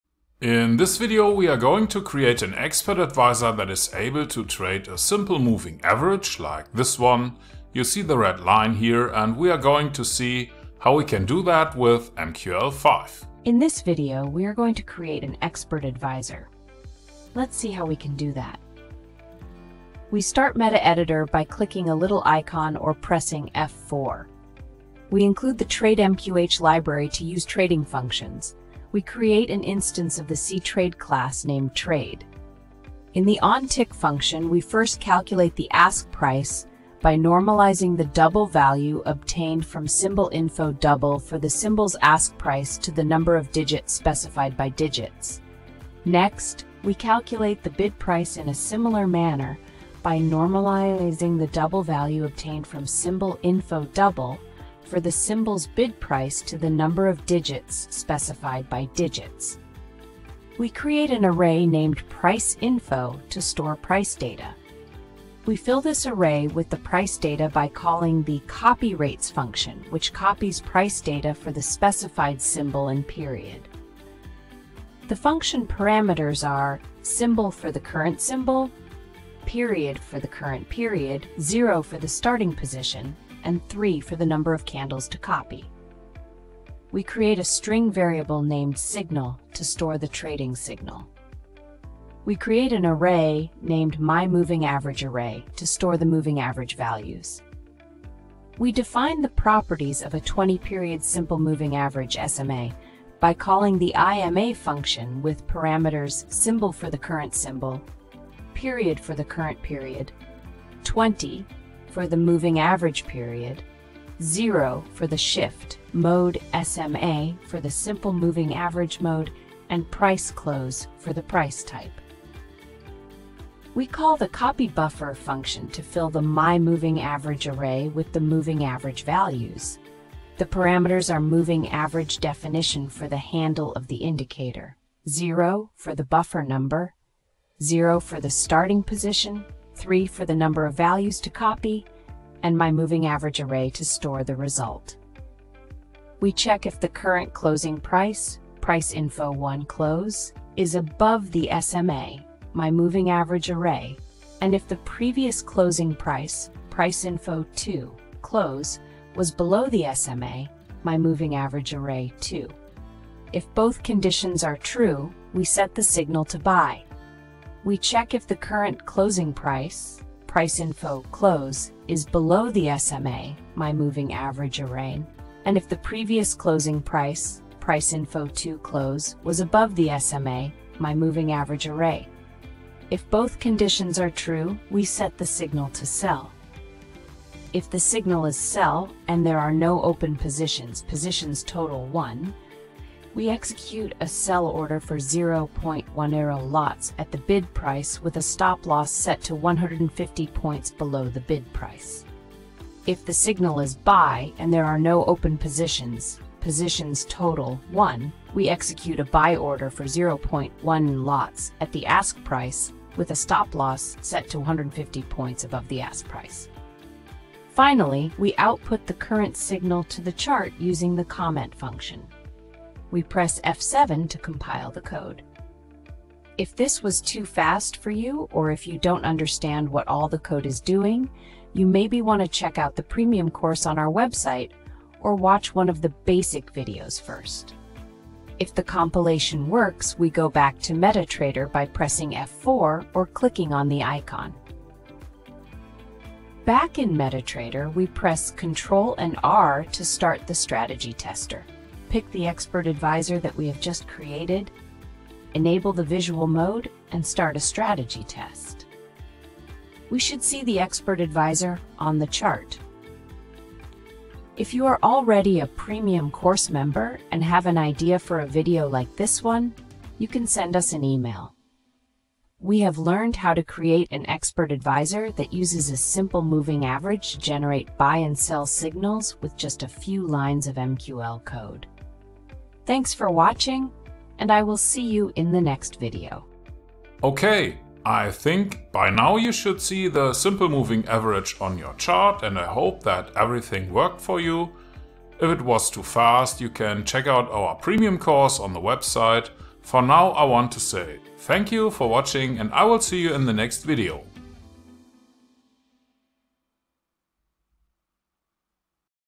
MQL5 TUTORIAL - SIMPLE MOVING AVERAGE EXPLAINED (in 5 minutes)
In this video, we are going to create an Expert Advisor. Let's see how we can do that.

We start Metaeditor by clicking a little icon or pressing F4.

We include the Trade mqh library to use trading functions. We create an instance of the CTrade class named trade.

In the On Tick function, we first calculate the Ask price by normalizing the double value obtained from Symbol Info Double for the symbol's ask price to the number of digits specified by Digits.

Next, we calculate the Bid price in a similar manner by normalizing the double value obtained from Symbol Info Double for the symbol's bid price to the number of digits specified by Digits.

We create an array named Price Info to store price data.

We fill this array with the price data by calling the Copy Rates function, which copies price data for the specified symbol and period. The function parameters are Symbol for the current symbol, Period for the current period, 0 for the starting position, and 3 for the number of candles to copy.

We create a string variable named signal to store the trading signal.

We create an array named my Moving Average Array to store the moving average values.

We define the properties of a 20-period simple moving average (SMA) by calling the iMA function with parameters Symbol for the current symbol, Period for the current period, 20 for the moving average period, 0 for the shift, MODE SMA for the simple moving average mode, and PRICE CLOSE for the price type.

We call the Copy Buffer function to fill the my Moving Average Array with the moving average values. The parameters are moving Average Definition for the handle of the indicator, 0 for the buffer number, 0 for the starting position, 3 for the number of values to copy, and my Moving Average Array to store the result.

We check if the current closing price (Price Info [1].close) is above the SMA (my Moving Average Array [1]) and if the previous closing price (Price Info [2].close) was below the SMA (my Moving Average Array [2]). If both conditions are true, we set the signal to buy.

We check if the current closing price (Price Info [1].close) is below the SMA (my Moving Average Array [1]) and if the previous closing price (Price Info [2].close) was above the SMA (my Moving Average Array [2]). If both conditions are true, we set the signal to sell.

If the signal is sell and there are no open positions (Positions Total () below 1), we execute a sell order for 0.10 lots at the Bid price with a stop loss set to 150 points below the Bid price.

If the signal is buy and there are no open positions (Positions Total () below 1), we execute a buy order for 0.10 lots at the Ask price with a stop loss set to 150 points above the Ask price.

Finally, we output the current signal to the chart using the Comment function.

We press F7 to compile the code.

If this was too fast for you or if you don't understand what all the code is doing, you maybe want to check out the Premium course on our website, or watch one of the basic videos first.

If the compilation works, we go back to MetaTrader by pressing F4 or clicking on the icon.

Back in MetaTrader, we press Control and R to start the strategy tester, pick the Expert Advisor that we have just created, enable the visual mode, and start a strategy test.

We should see the Expert Advisor on the chart.

We have learned how to create an Expert Advisor that uses a simple moving average to generate buy and sell signals with just a few lines of MQL code.